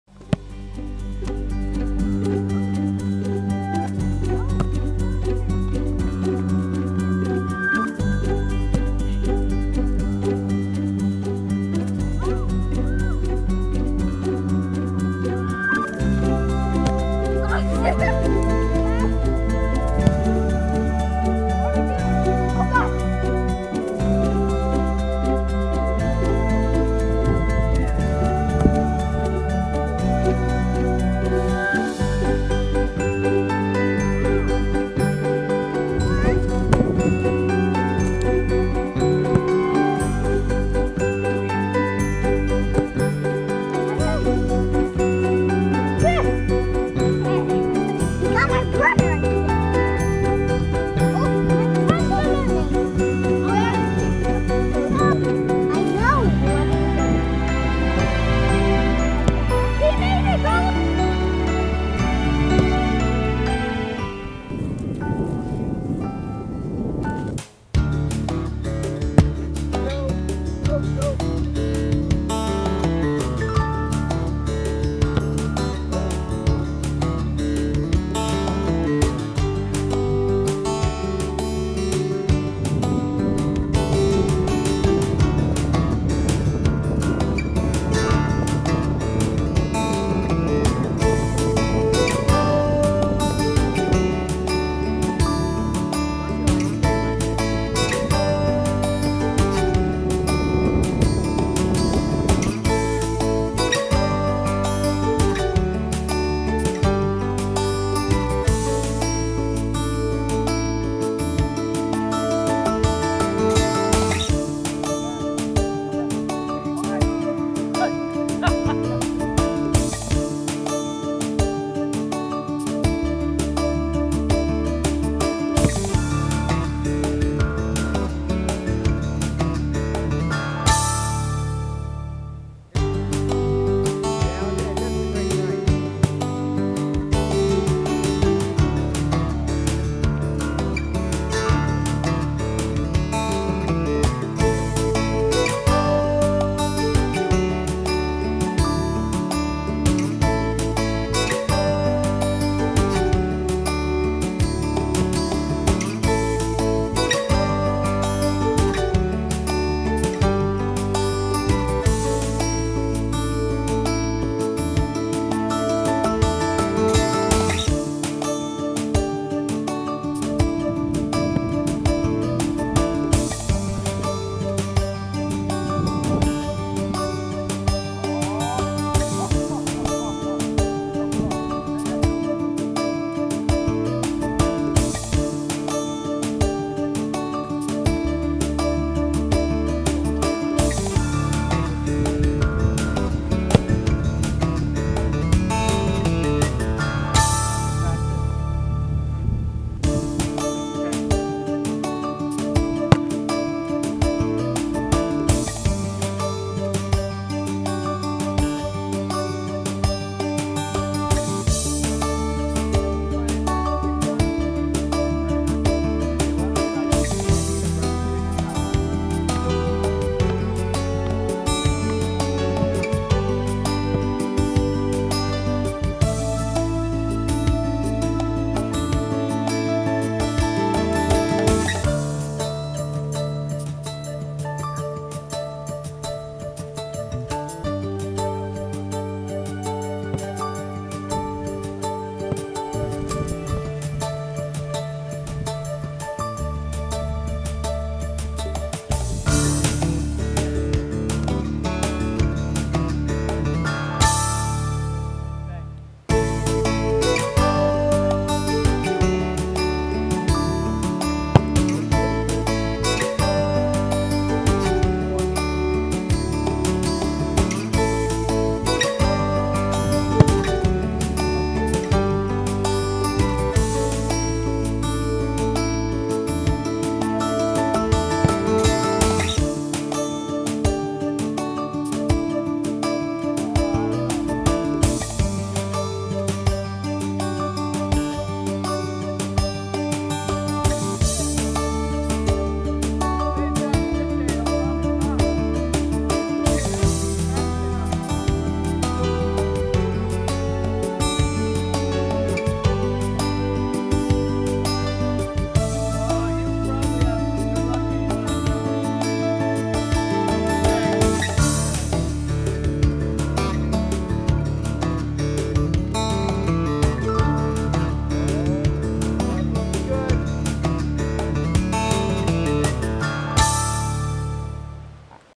FootGolf1QRF YouTube HD 1080p 001SQ
FootGolf opening at Oconee Country Club-first cut. November 2014, Seneca, SC  5:25 minutes